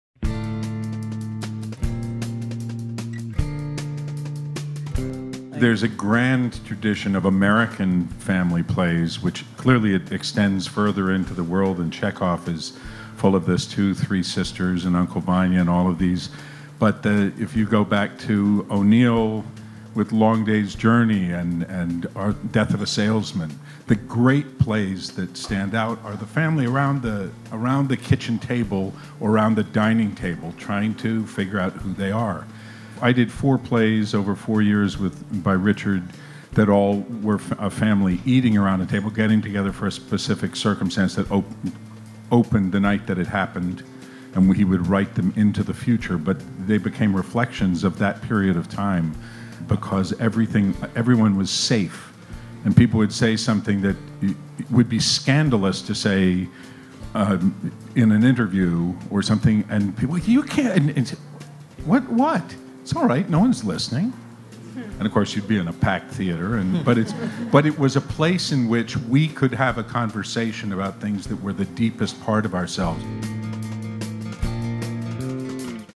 PUBLIC FORUM: Jay O. Sanders on family stories
Jay O. Sanders, patriarch of both Richard Nelson's Apple and Gabriel families, told Public Forum about the specific joy of those plays: bringing family onto the stage.

PUBLIC FORUM: The Long Christmas Dinner by Thornton Wilder
December 10, 2017 in Joe's Pub

film by Oresti Tsonopoulos